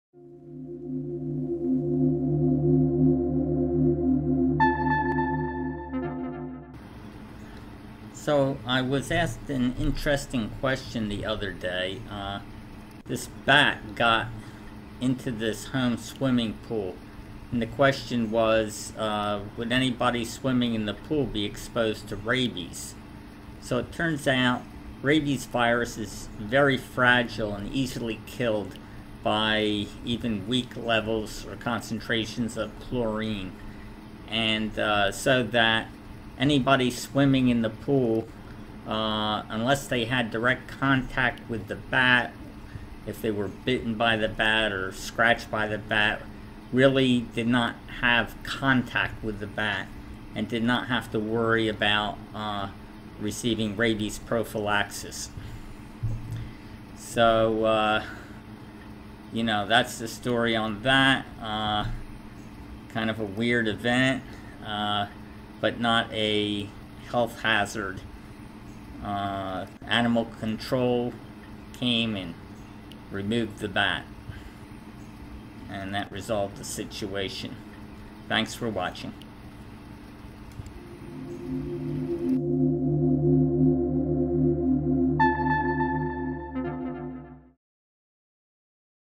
Bat in Swimming Pool
Does a bat in a swimming pool pose a rabies risk to people who swam in that pool?